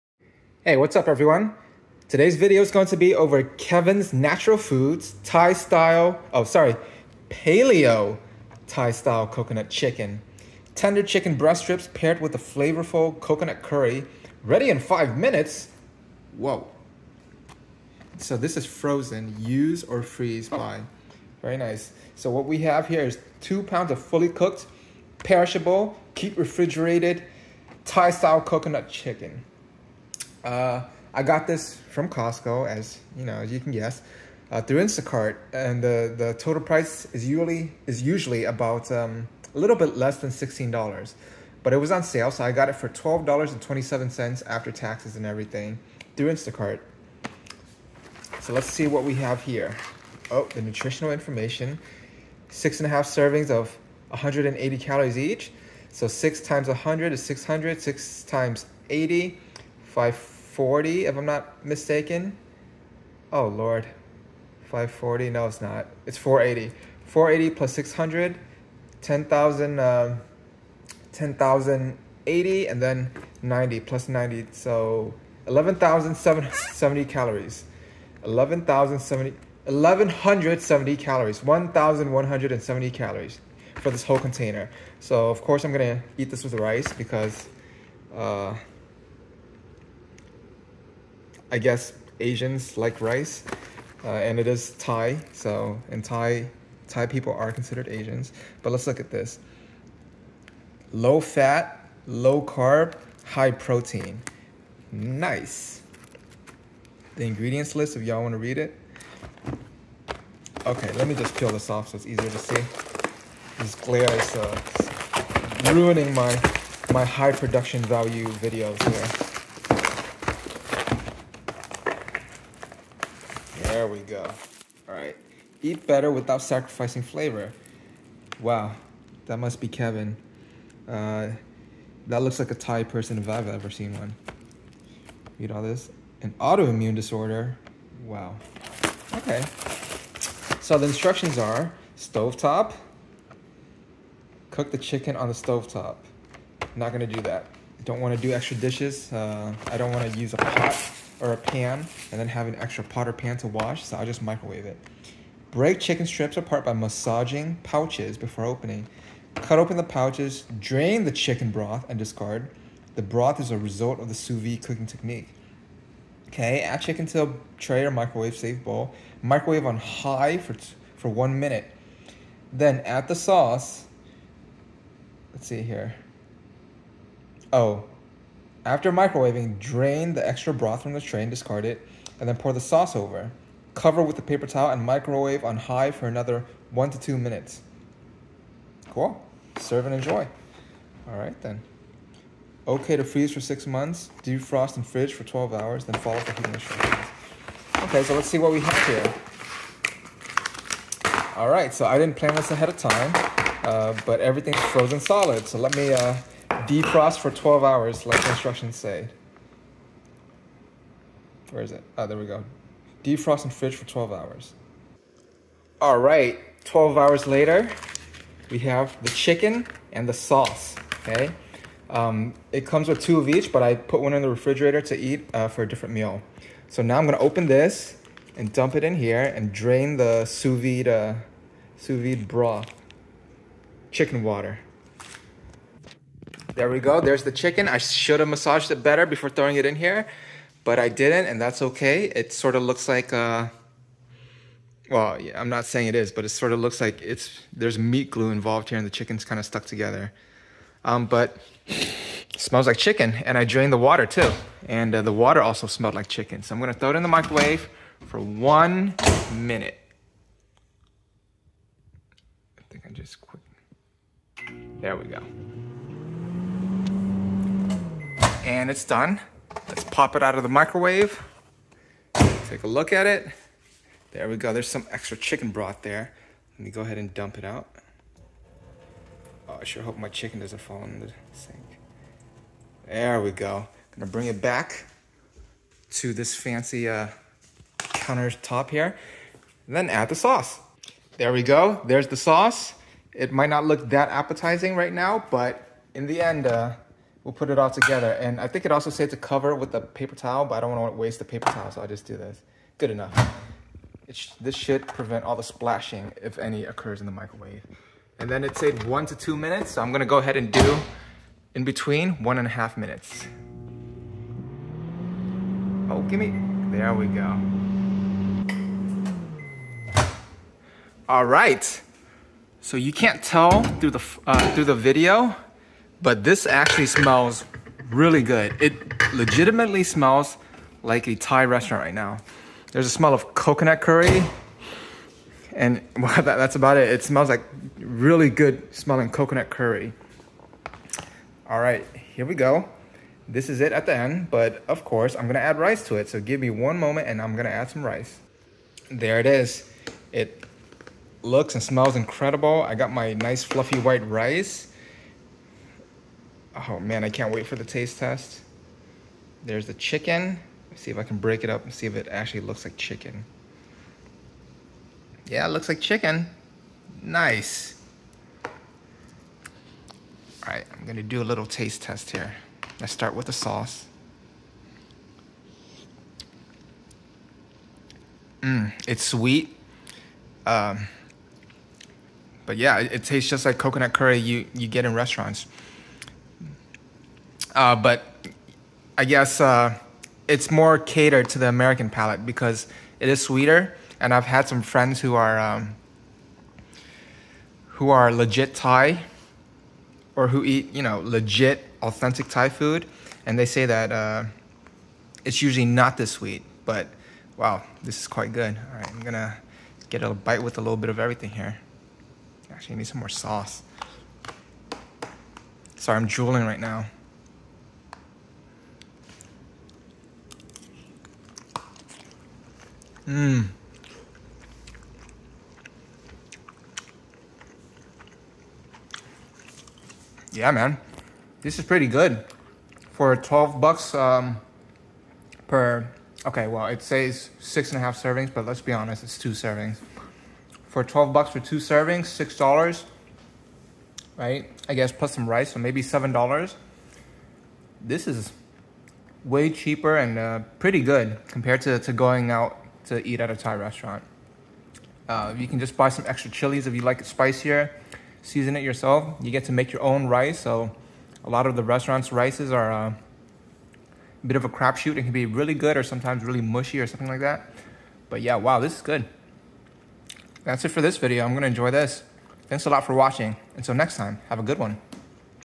Kevin's Natural Foods - Paleo Thai Style Coconut Chicken
Kevin's Natural Foods - Paleo Thai Style Coconut Chicken. Tender chicken breast strips paired with a flavorful coconut curry. This mild but flavorful curry will make a great meal for the entire family! Tender chicken breast strips in a rich and creamy blend of coconut milk, basil and Thai spices. From Costco.